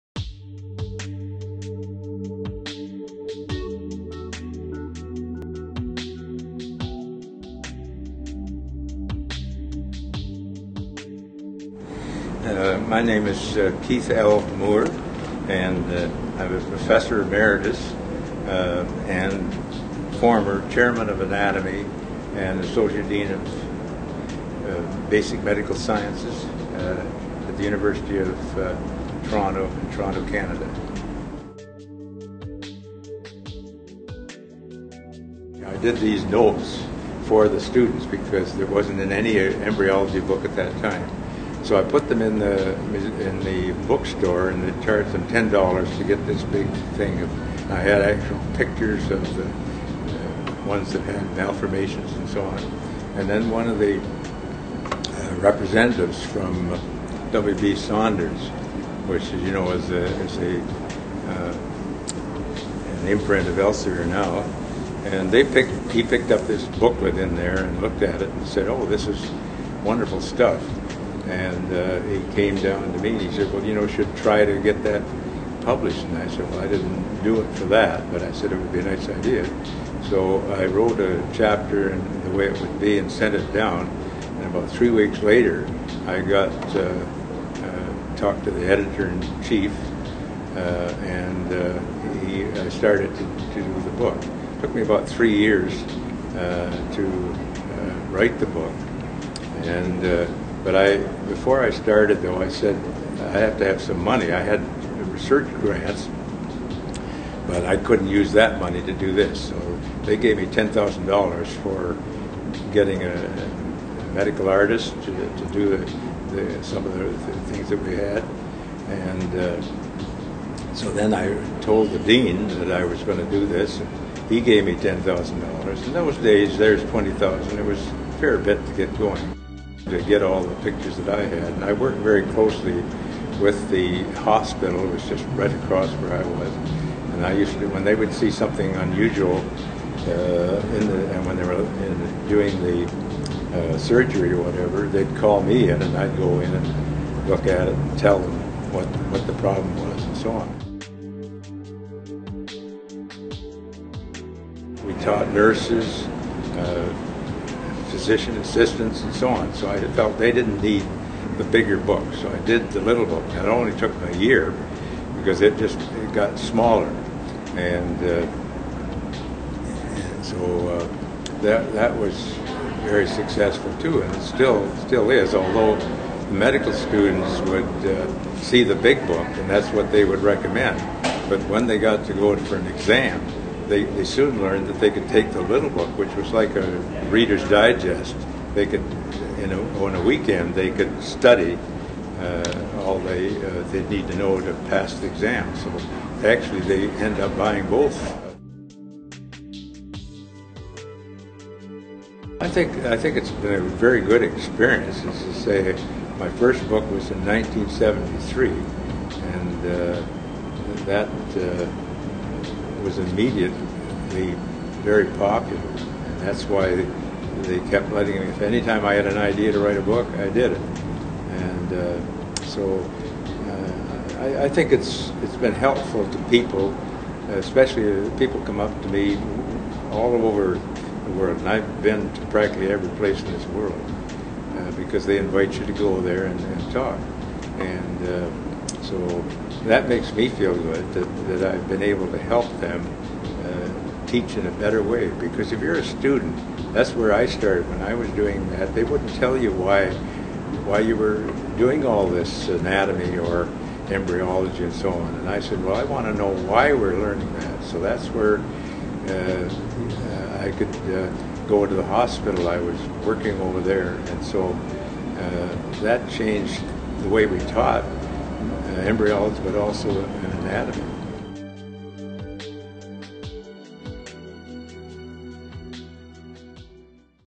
Dr. Keith Moore, an Elsevier Author, at EB 2012
Dr. Keith Moore discusses the origin of his book "The Developing Human," currently in its 9th edition, as well as "Before We Are Born," currently in its 8th edition.

"Before We Are Born, 8th Edition."

To learn more about Dr. Moore, please visit his official Elsevier Authors